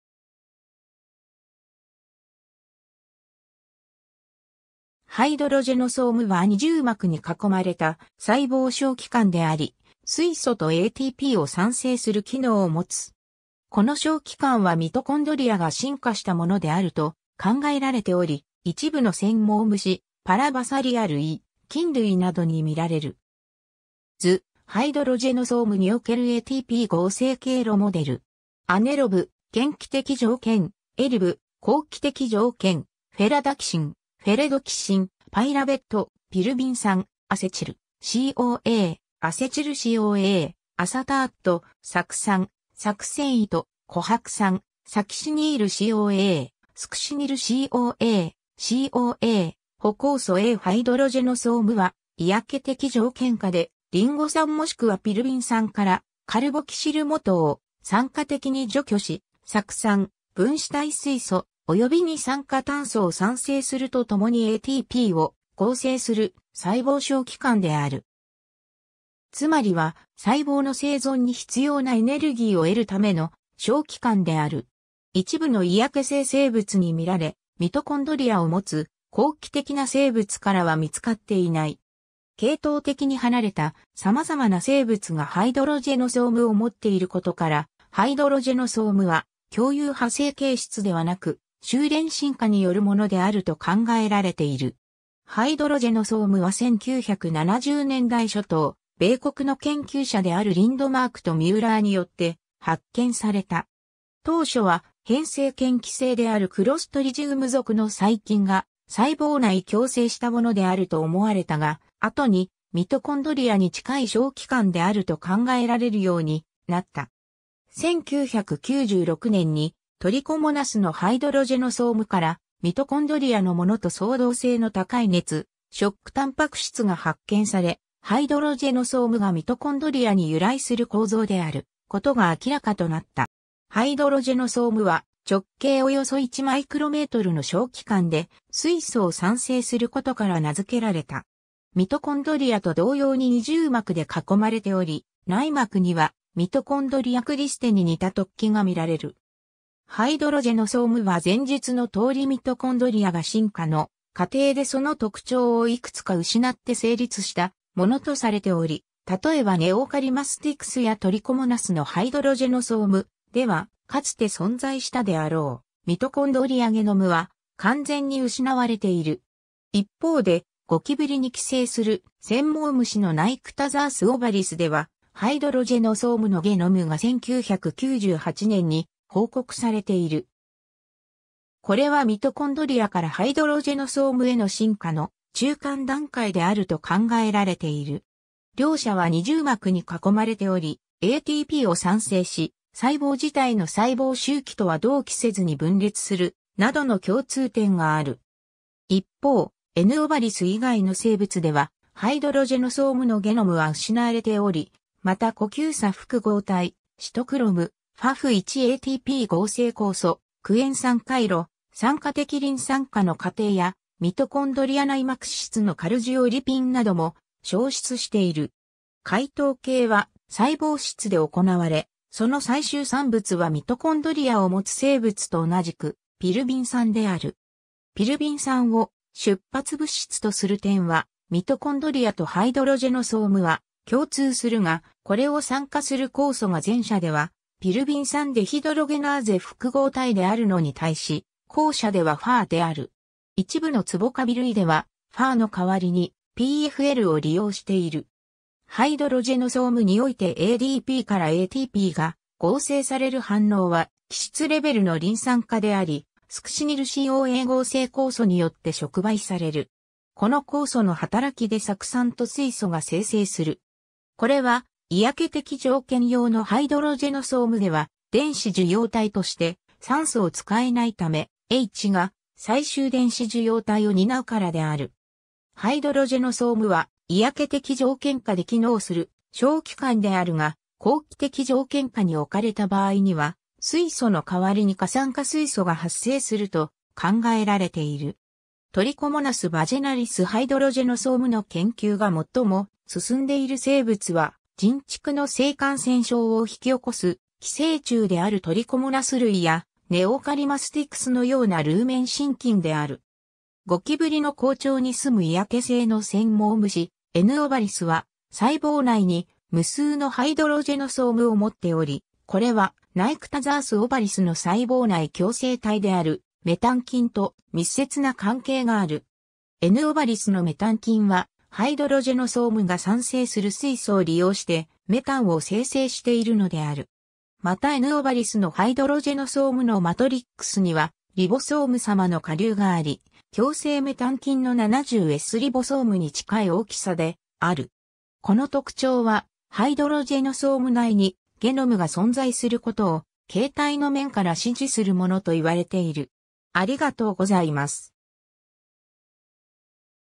ハイドロジェノソーム
ハイドロジェノソーム（ヒドロゲノソーム、Hydrogenosome）は二重膜に囲まれた細胞小器官であり、水素とATPを産生する機能を持つ。
この小器官はミトコンドリアが進化したものであると考えられており、一部の繊毛虫、パラバサリア類、菌類などに見られる。
 図　ハイドロジェノソームにおけるATP合成経路モデル. aneaerob：嫌気的条件、aerob：好気的条件、Ferredoxin：フェレドキシン、Pyruvat：ピルビン酸、Acetyl-CoA：アセチルCoA、Acetat：酢酸、Succinat：コハク酸、Succinyl-CoA：スクシニルCoA、CoA：補酵素A ハイドロジェノソームは嫌気的条件下でリンゴ酸もしくはピルビン酸からカルボキシル基を酸化的に除去し、酢酸、分子態水素、および二酸化炭素を産生するとともに ATP を合成する細胞小器官である。
つまりは細胞の生存に必要なエネルギーを得るための小器官である。
一部の嫌気性生物に見られ、ミトコンドリアを持つ好気的な生物からは見つかっていない。
系統的に離れた様々な生物がハイドロジェノソームを持っていることから、ハイドロジェノソームは共有派生形質ではなく、収斂進化によるものであると考えられている。
 ハイドロジェノソームは1970年代初頭、米国の研究者である Lindmark と Müller によって発見された。
当初は偏性嫌気性であるクロストリジウム属の細菌が細胞内共生したものであると思われたが、後にミトコンドリアに近い小器官であると考えられるようになった。
1996年にトリコモナス（Trichomonas vaginalis）のハイドロジェノソームからミトコンドリアのものと相同性の高い熱ショックタンパク質が発見され、ハイドロジェノソームがミトコンドリアに由来する構造である事が明らかとなった。
 ハイドロジェノソームは直径およそ1μmの小器官で、水素（Hydrogen）を産生することから名付けられた。
ミトコンドリアと同様に二重膜で囲まれており、内膜にはミトコンドリアクリステに似た突起が見られる（クリステが無い場合もある）。
ハイドロジェノソームは前述の通りミトコンドリアが進化の過程でその特徴を幾つか失って成立したものとされており、例えばネオカリマスティクス（Neocallimastix）やトリコモナス（T. vaginalis, T. foetus）のハイドロジェノソームでは、かつて存在したであろうミトコンドリアゲノムは完全に失われている。
一方で、ゴキブリに寄生する繊毛虫の Nyctotherus ovalis ではハイドロジェノソームのゲノムが1998年に報告されている。
これはミトコンドリアからハイドロジェノソームへの進化の中間段階（ミッシングリンク）であると考えられている。
両者は二重膜に囲まれており、ATPを産生し、（多くの生物では）細胞自体の細胞周期とは同期せずに分裂する、などの共通点がある。
一方、N. Ovalis 以外の生物ではハイドロジェノソームのゲノムは失われており、また呼吸鎖複合体、シトクロム、FoF1-ATP 合成酵素、クエン酸回路、酸化的リン酸化の過程やミトコンドリア内膜脂質のカルジオリピン（Cardiolipin）なども消失している。
 解糖系は細胞質で行われ、その最終産物はミトコンドリアを持つ生物と同じくピルビン酸である。
ピルビン酸を出発物質とする点はミトコンドリアとハイドロジェノソームは共通するが、これを酸化する酵素が前者ではピルビン酸デヒドロゲナーゼ複合体であるのに対し、後者では PFOR（Pyruvate:Ferredoxin Oxidoreductase）である。
一部のツボカビ類では PFOR の代わりに PFL（Pyruvate Formate Lyase、ピルビン酸ギ酸リアーゼ）を利用している。
 ハイドロジェノソームにおいて ADP から ATP が合成される反応は基質レベルのリン酸化であり、スクシニルCoA合成酵素によって触媒される。
この酵素の働きで酢酸と水素が生成する。
これは、嫌気的条件用のハイドロジェノソームでは電子受容体として酸素を使えないため、H+が最終電子受容体を担うからである。
ハイドロジェノソームは嫌気的条件下で機能する小器官であるが、好気的条件化に置かれた場合には水素の代わりに過酸化水素が発生すると考えられている。
 Trichomonas vaginalis ハイドロジェノソーム...